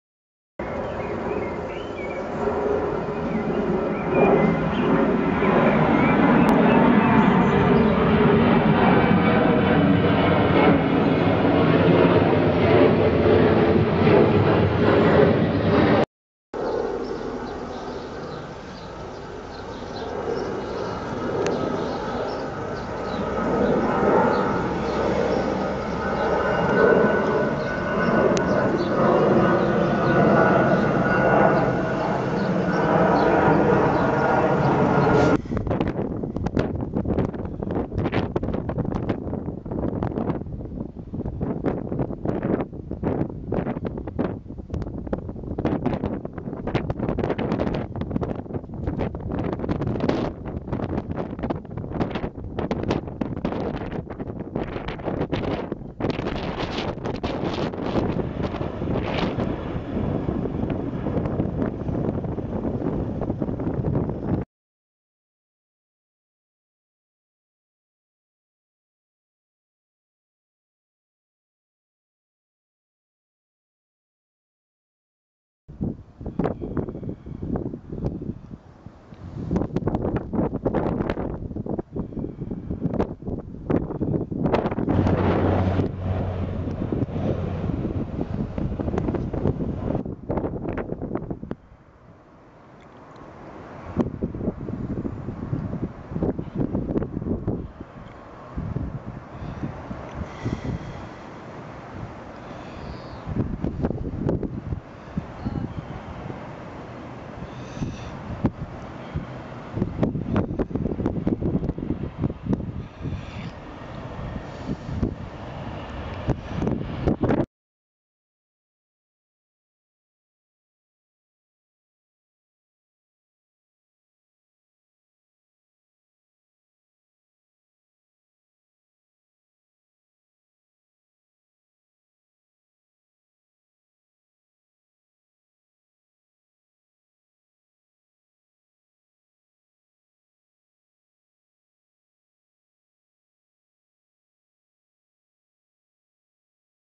Leeds Bradford Airport. (A day out)
Short vid of my visit to Leeds Bradford Airport...Included a few stills with a nice shot of a very tight bank to the left !